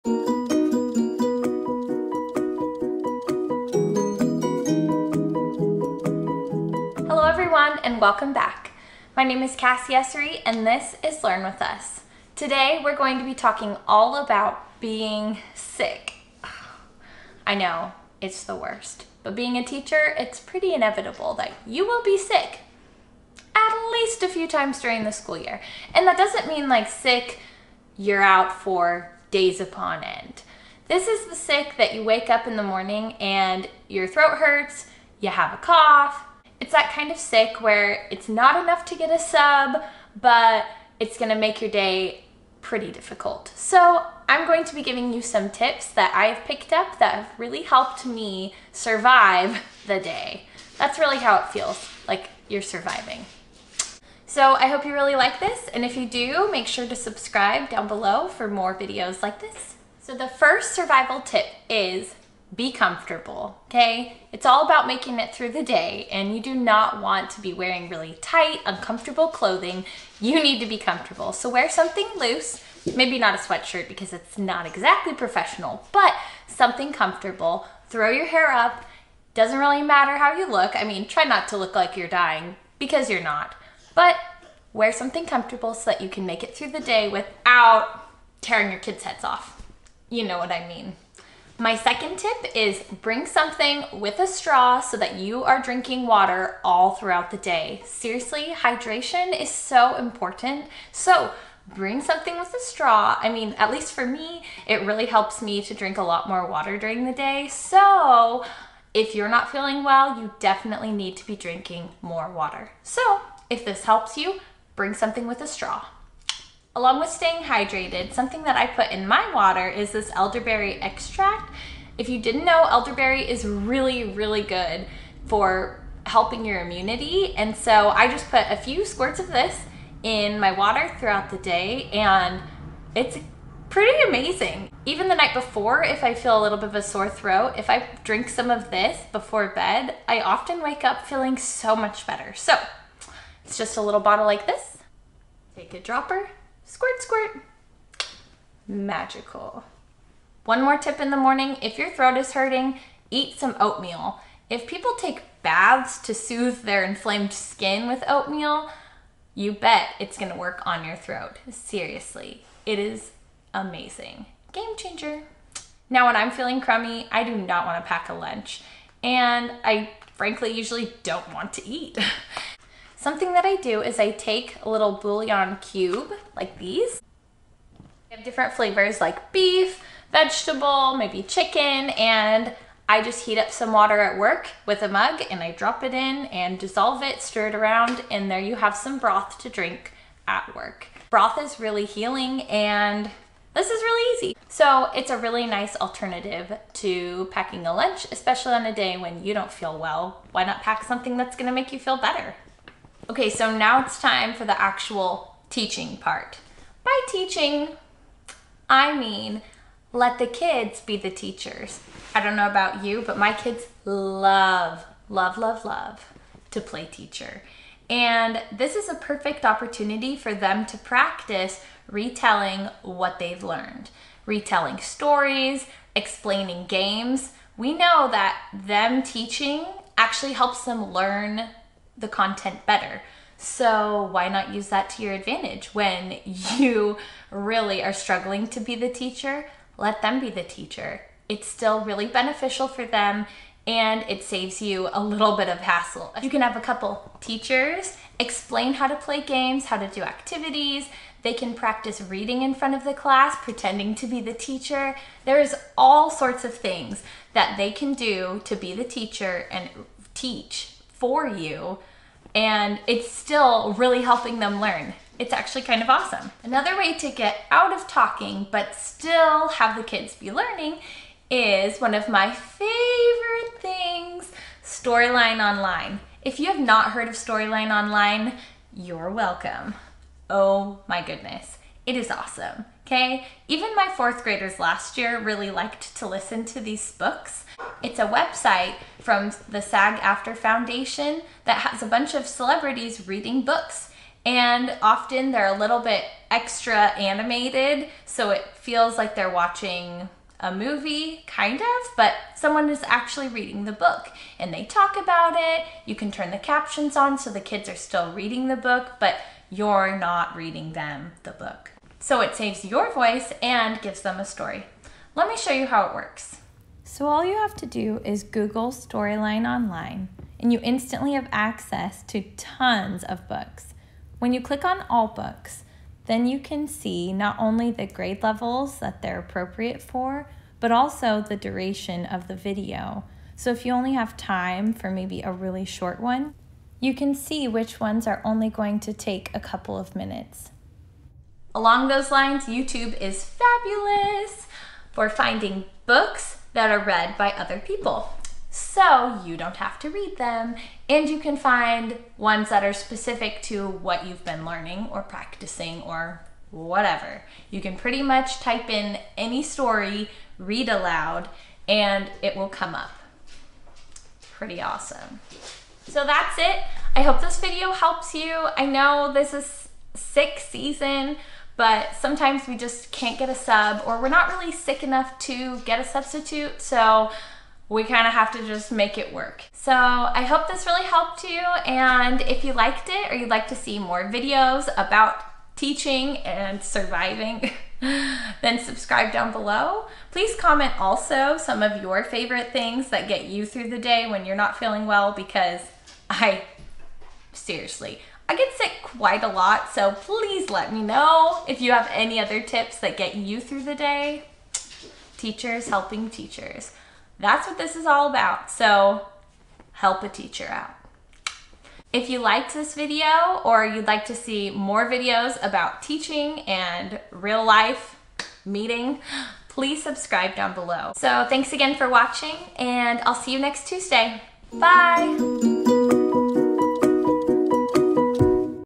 Sick At School: How To Survive The Day
If you’ve ever even been NEAR a teacher, you know that we get sick quite often. This video is for you teachers and full of tips to help you get through the day, and your kids will still be learning! Comment any tricks you have to help us all during cold season.

Camera Equipment:

Sony a7ii :

Ring Light:

Editing Equipment:

iPad Pro:

Apple Pen 2nd Gen: